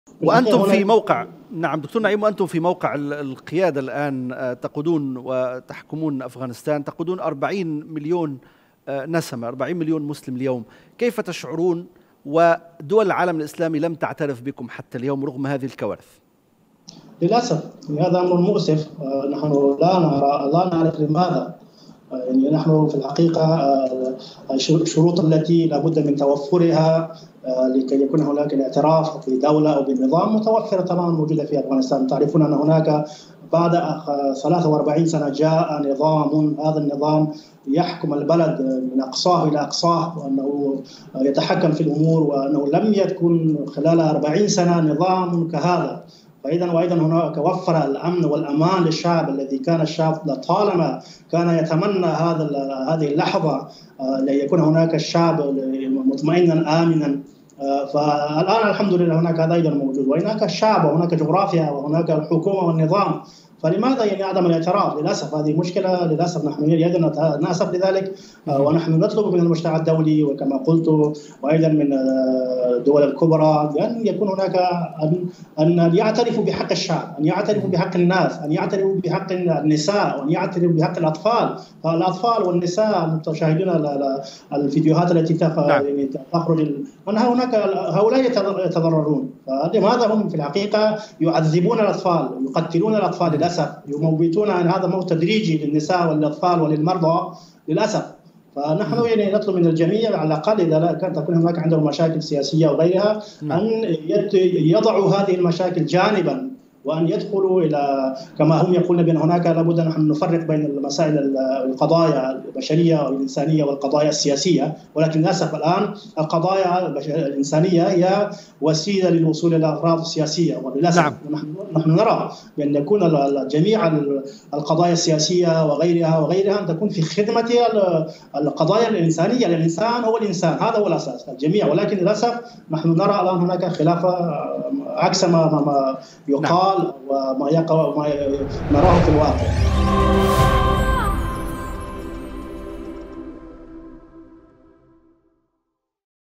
شاهد المتحدث باسم طالبان يعلن أسفه من عدم اعتراف العالم الإسلامي ويرسل رسالة للعالم
Nilesat (النايل سات): 11603 أفقي